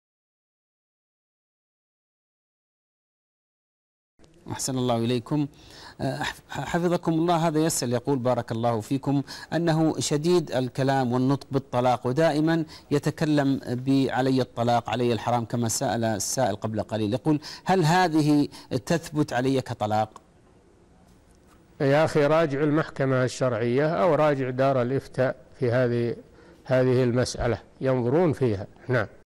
رجل يكثر من قول (عليَّ الطلاق)، فما الحكم؟ - الشيخ صالح الفوزان
نص السؤال: هذا يسأل يقول بارك الله فيكم: أنه شديد الكلام والنطق بالطلاق، ودائماً يتكلم بـ "عليّ الطلاق"، "عليّ الحرام"، هل هذه تثبت عليّ كطلاق؟
السيرة الذاتية :
للاشتراك في قناة الشيخ صالح الفوزان
لمشاهدة حلقات فتاوى على الهواء كاملة

رئيس الفريق
لمشروع كبار العلماء